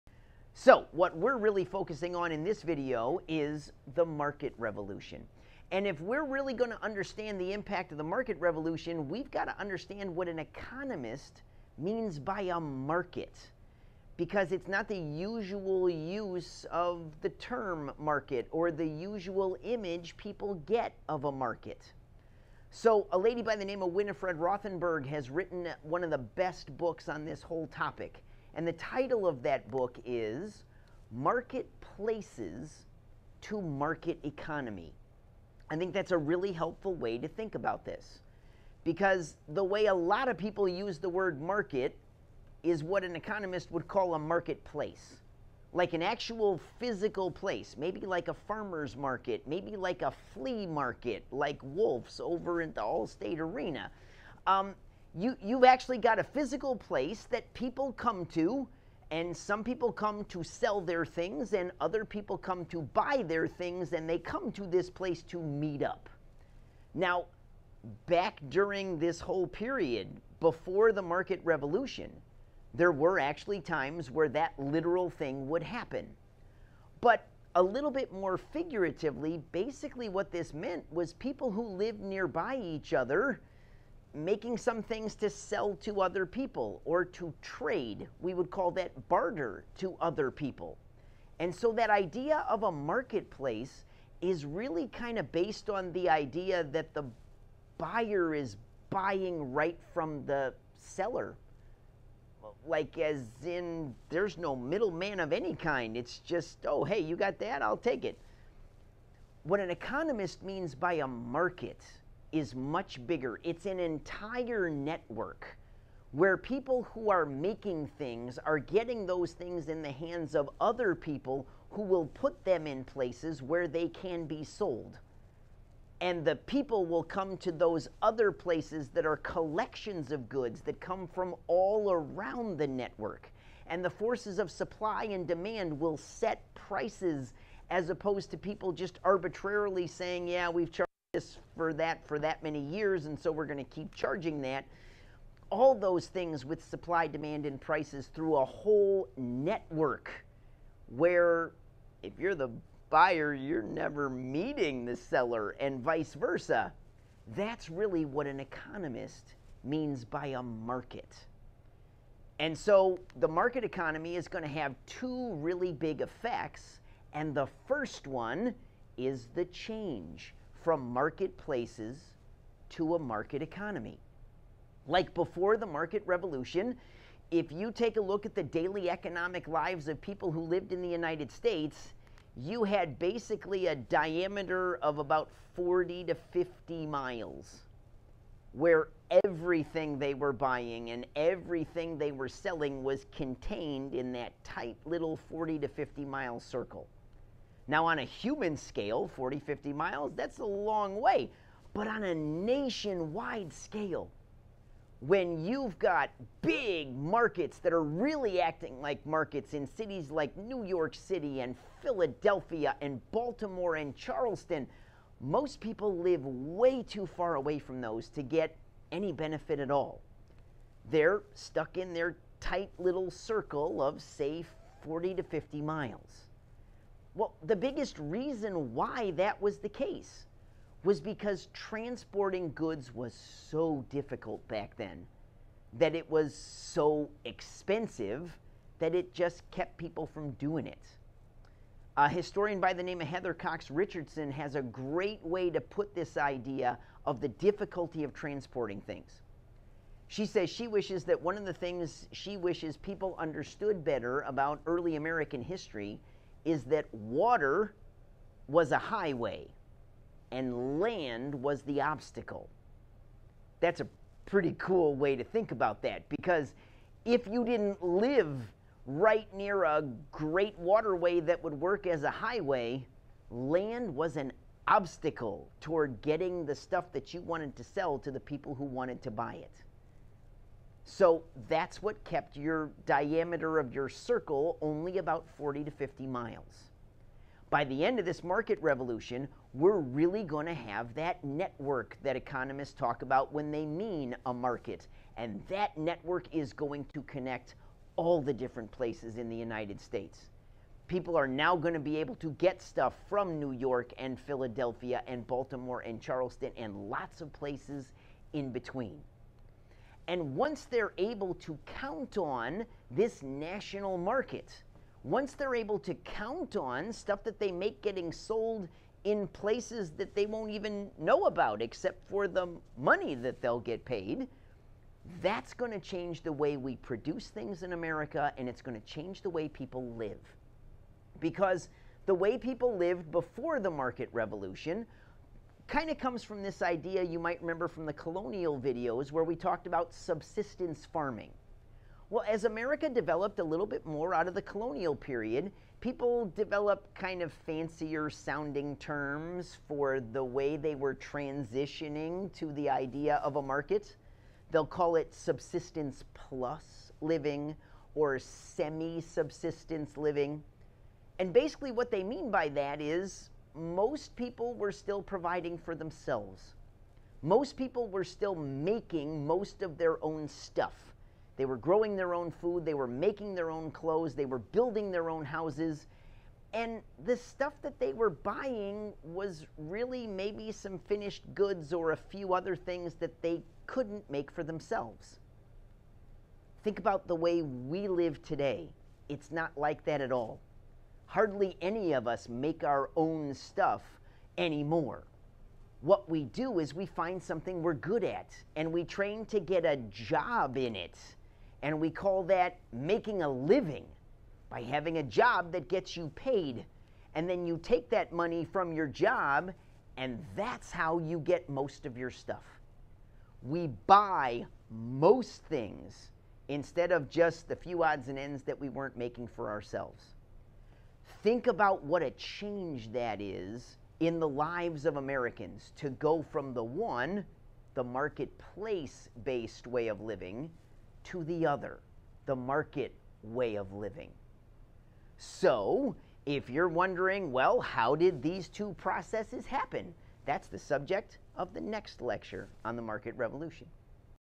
Market Revolution - The Effects
In which we discuss the biggest effects on the everyday lives of Americans that came from the Market Revolution